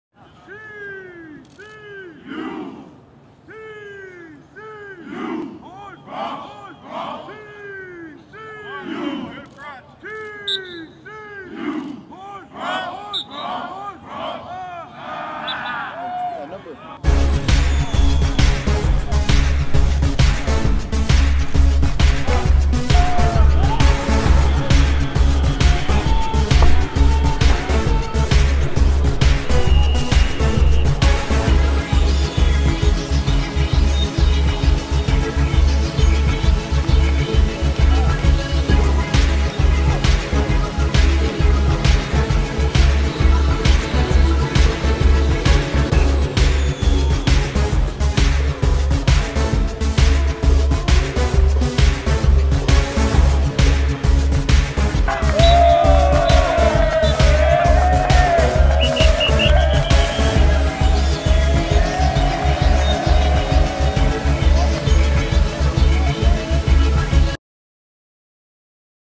Sights And Sounds From TCU's Alamo Bowl Practice
Sights and sounds from TCU's first practice at The University of Incarnate Word in San Antonio in preparation for the Jan. 2 Alamo Bowl. (Video: Travis L. Brown)